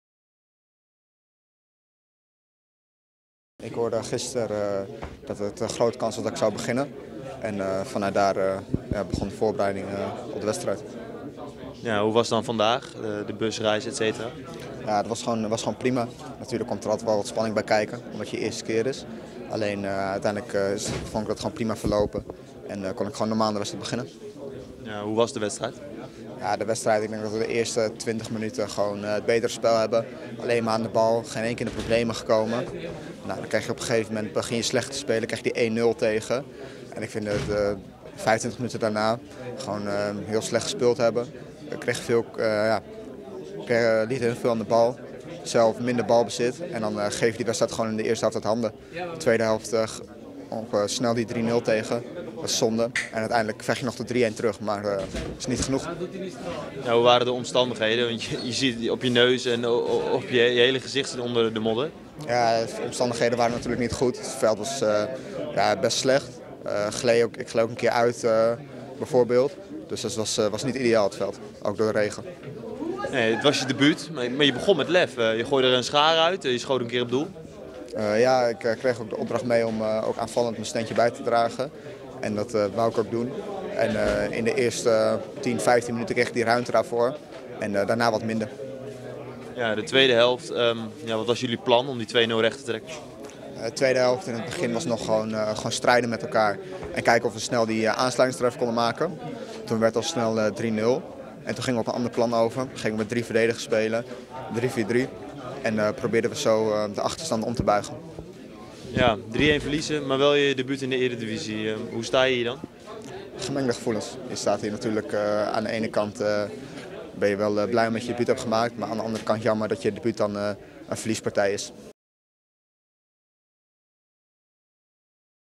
Thomas Poll heeft gemengde gevoelens na debuut
FC Groningen heeft vrijdagavond in de 25ste speelronde van de Eredivisie met 3-1 verloren op bezoek bij Willem II. Thomas Poll debuteerde voor FC Groningen in de Eredivisie.